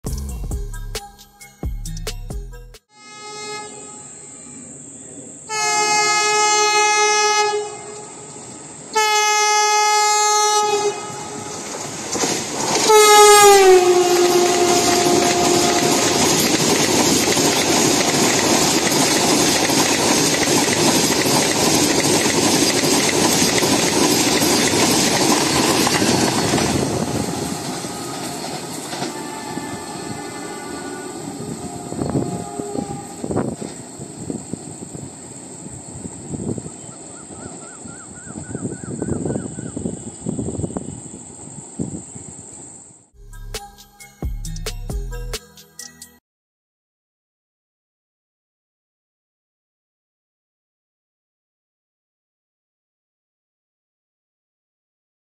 Vande Bharat Express 120KM/H skip || Chennai VBE || NomadGRJ || 4K •••
took quite the ride! had to change three trains to come and get this shot all the way from home, yikes!
for those wondering, this is MYS-MAS VBE, with the number 20608. this service uses a 16 coach VB trainset, with a maximum design speed of 200, maximum tested speed of 180, and maximum line permissable speed of 130. the train in this shot is moving at about 120, due to the speed limit of -10 from MPS at this section of track.

im on Instagram Threads as well now, same ID